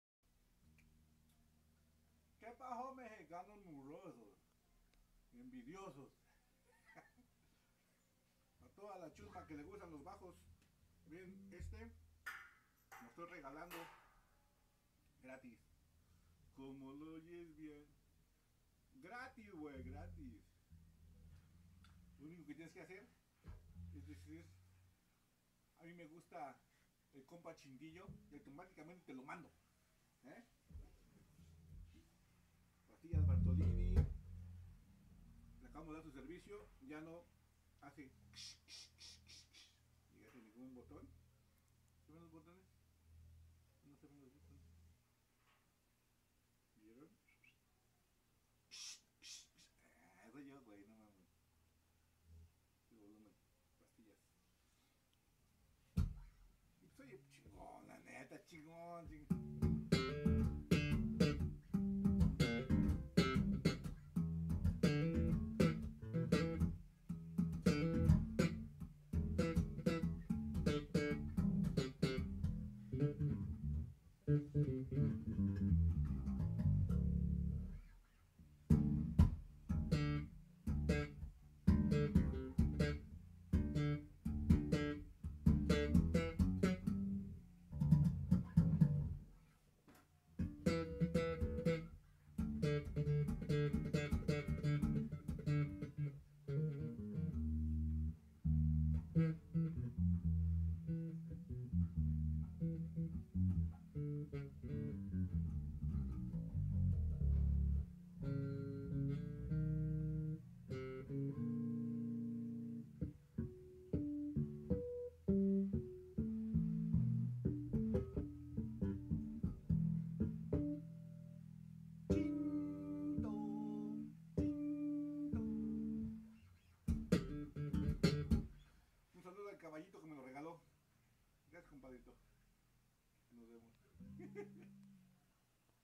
limpieza de Bass
Probando bass  despues de haberle dado su mantenimiento correctivo y preventivo